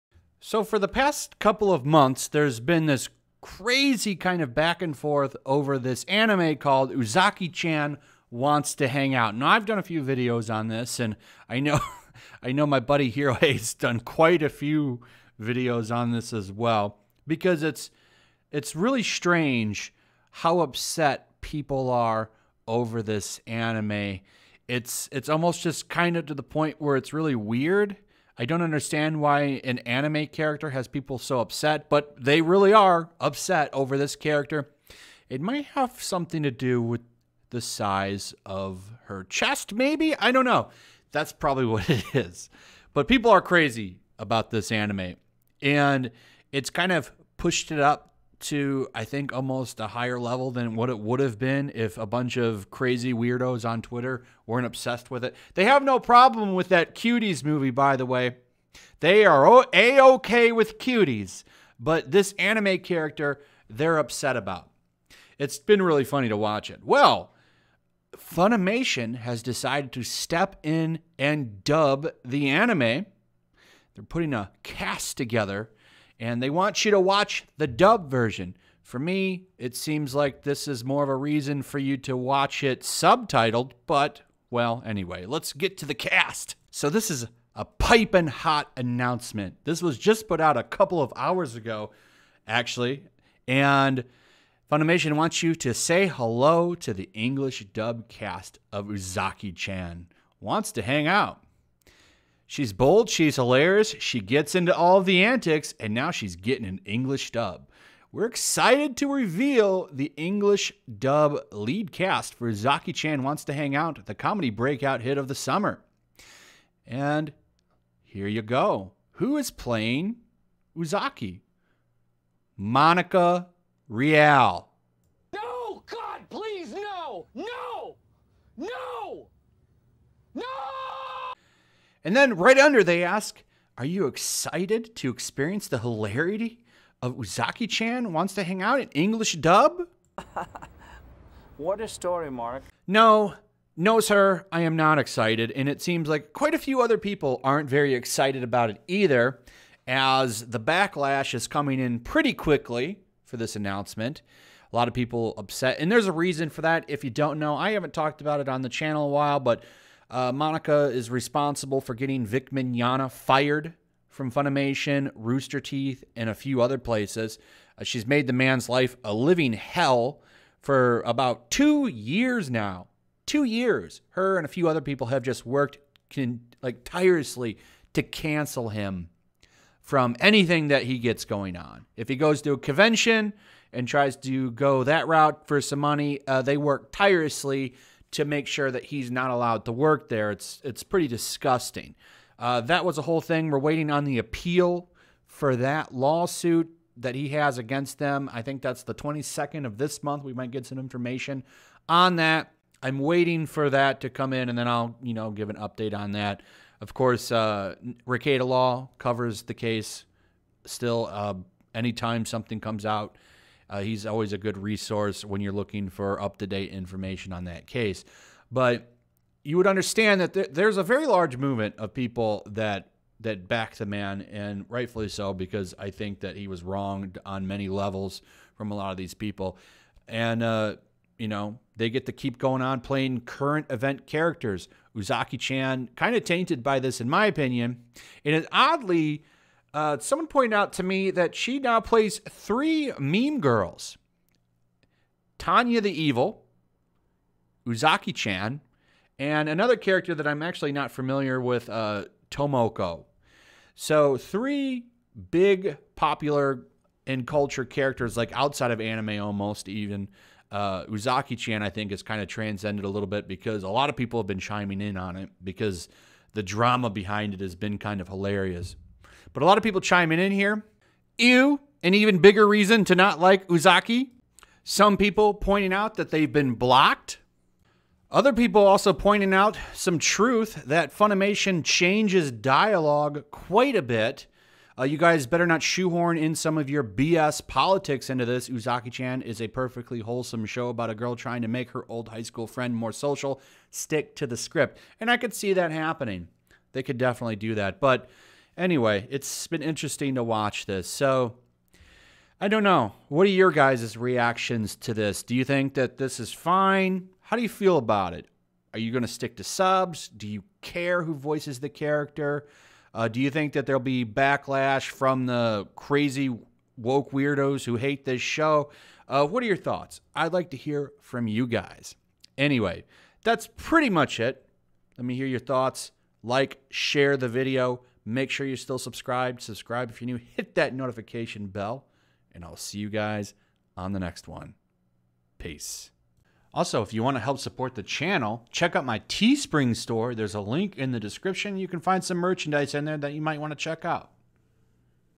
Funimation is ready to ruin Uzaki-Chan! Will they hurt her with their politics and cast!?
Uzaki-Chan has been picked up by Funimation for a dub, and they picked the worst possible person to voice her! Will they ruin the show with their horrible political agenda?